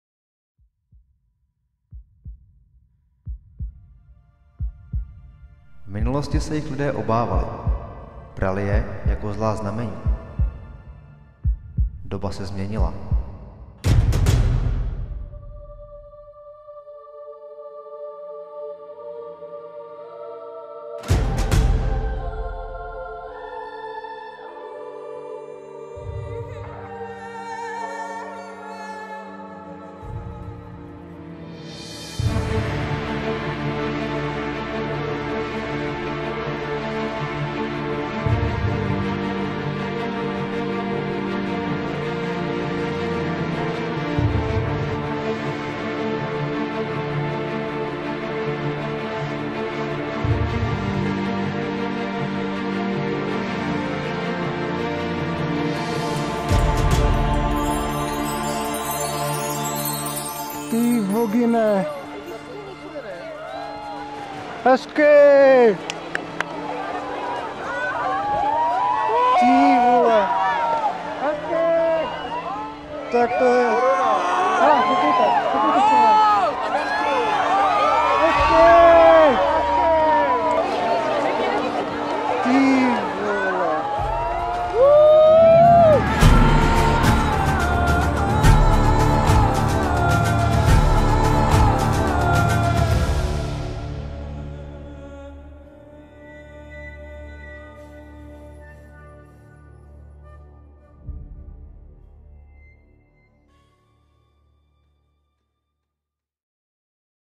Tajemná zatmění (upoutávka ke knize)
"Putování po celém světě za nejkrásnějšími nebeskými divy."

V minulosti se jich lidé obávali. Brali je jako zlá znamení. Doba se však změnila… Nové, doplněné vydání knihy známého fotografa hvězdné oblohy Petra Horálka bude vaším průvodcem po patrně nejkrásnějších a zároveň jedněch z nejvzácnějších úkazů na obloze – slunečních a měsíčních zatměních. Dozvíte se, jak bylo lidstvo od nepaměti fascinováno těmito jevy, poznáte historii pozorování i vědeckou současnost. Poznáte zákony, podle kterých se úkazy zatmění řídí a co z nich činí nezapomenutelné zážitky.  Knihu získáte na pultech českých a slovenských knihkupectví, nebo online na webu nakladatelství

Střih, režie: Petr Horálek
Autoři použitého vizuálu: Petr Horálek, Miloslav Druckmüller a kol., Luis Calçada/ESO, Discovery Network, Kuroda Terutoshi, Itaru Takuka, Webcamsdemexico, Albatrosmedia
Hudba: Dexter Britain, Jan Morgenstern